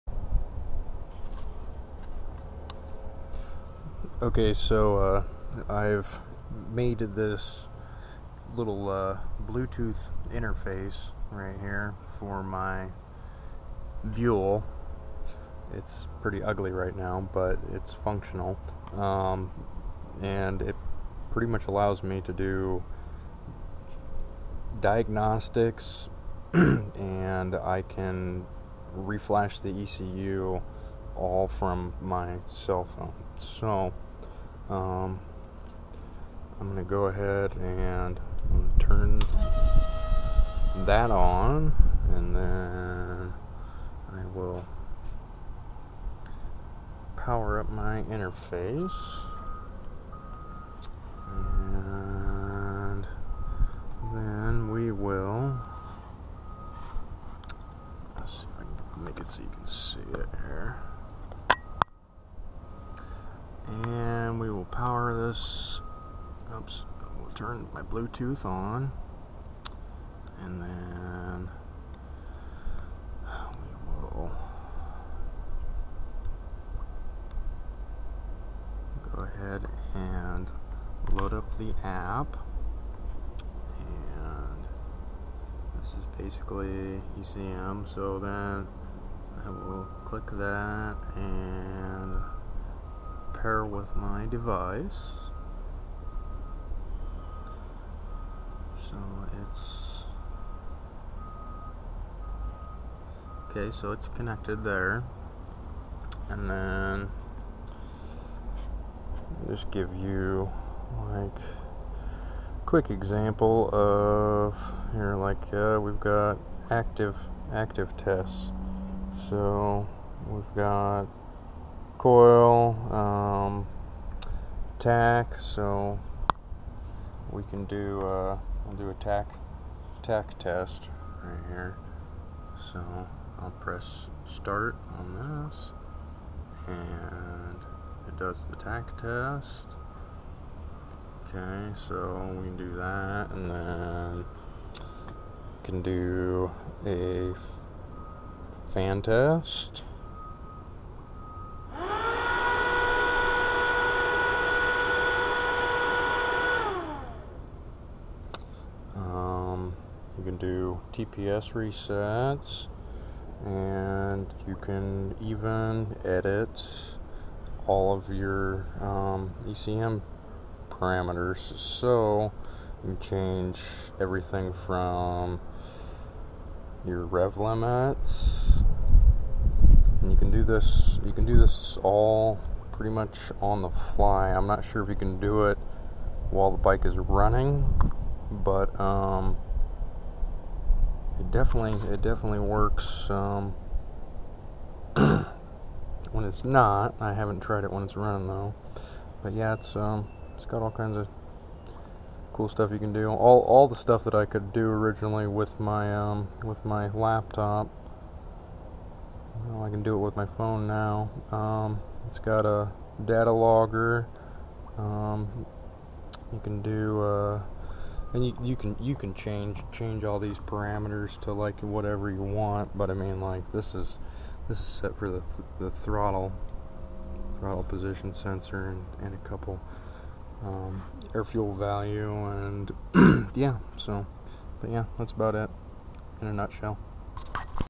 Buell XB ECM Tuning
Looking for an android ELM diagnostic APP I happened to stumble on  ECMDROID. I had made and interface years ago so I could tune the bike with ECMSPY. So, I made a Bluetooth interface on a whim from junk in the lab. But this is just a quick demo of the  APP a